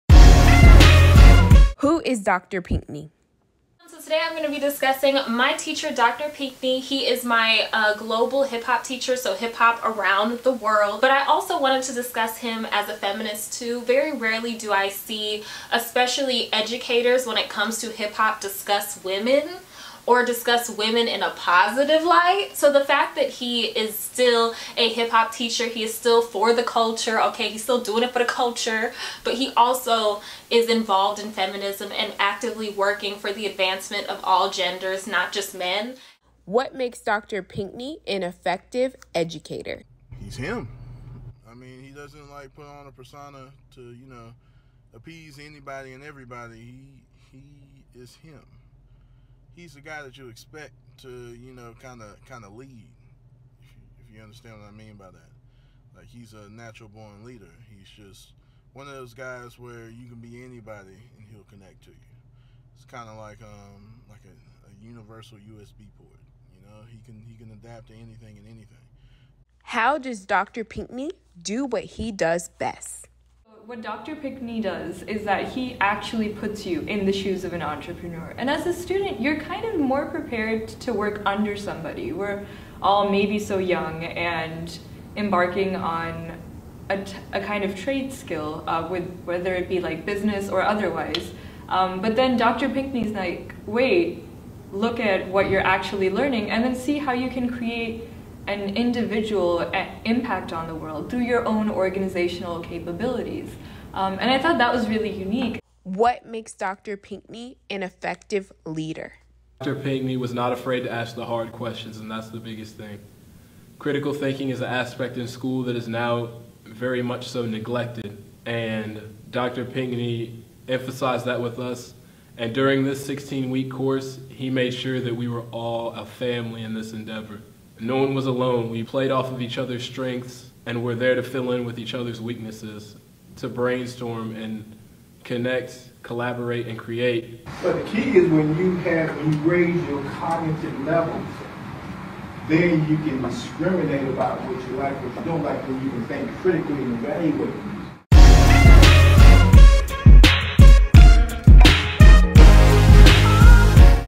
Dr. Charles Pinckney's Introduction Video
As a Hip-Hop Professor, Dr. Charles Pinckney's former students highlight his work as an educator.

Purchase his book, entitled "From Slaveships to Scholarships: The Plight of the American Athlete" on Amazon